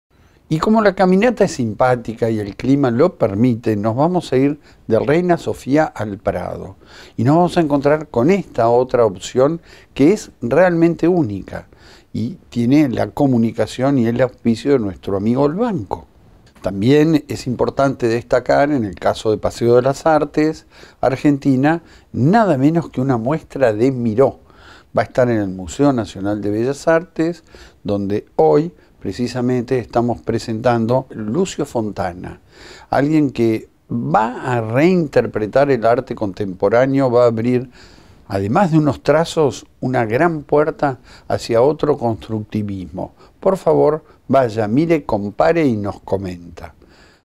Paseo de las Artes - Tesoros de la Hispanic Society, Miró y Lucio Fontana en el Bellas Artes
3 eventos esta semana
- seguimos recomendando la muestra de los tesoros de la Hispanic Society en el Museo Reina Sofía de Madrid
- el Museo Nacional de Bellas Artes anunció una muestra retrospectiva de la obra de Joan Miró en octubre y noviembre.
- y para terminar también en el MNBA abrió la muestra "Lucio Fontana en las colecciones públicas de la Argentina".
Curada por Andrés Duprat y Fernando Farina, la muestra recorre la trayectoria del artista ítalo-argentino desde sus esculturas tempranas figurativas, hasta sus experimentaciones en torno a los denominados “Conceptos espaciales”, con los que proponía una expansión de la bidimensionalidad de la pintura.
La muestra inaugura el viernes 28 a las 19, en la sala 33 del Museo. Podrá visitarse hasta el 30 de julio, con entrada libre y gratuita.